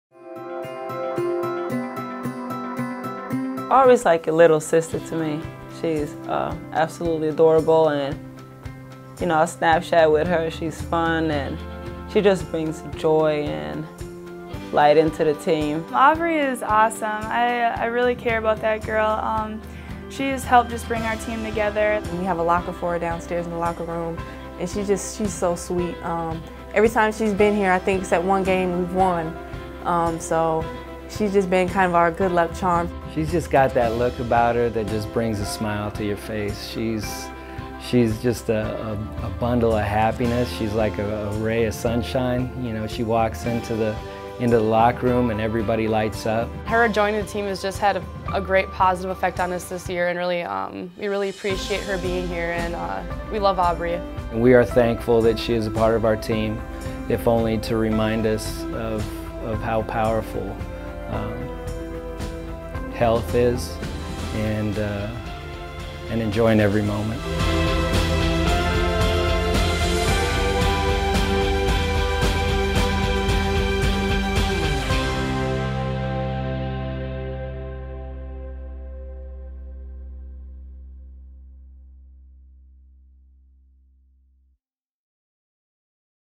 Aubrey King: Honorary Golden Gopher
Years after signing up to adopt a child through the Friends of Jaclyn Foundation, the University of Minnesota women's basketball team finally received the news in early 2014 that they were paired up with Aubrey King, a 12-year-old from Chaska who has been living with juvenile pilocytic astrocytoma, a pediatric brain tumor, since she was three years old.

Aubrey was diagnosed with a brain tumor in 2005, at the age of three, and shortly after had surgery to remove part of the growth. Over the last eight years, she has been on and off with four different chemotherapy regimens, most recently starting her fourth in May at the Mayo Clinic.

Through the surgery performed in 2005, doctors were able to remove about half of her tumor. Nerve damage from surgery left her with hearing loss in her left ear, a paralyzed left vocal cord and a partially paralyzed palette. Though they are minor problems in the big picture, she will have to manage these losses throughout her life.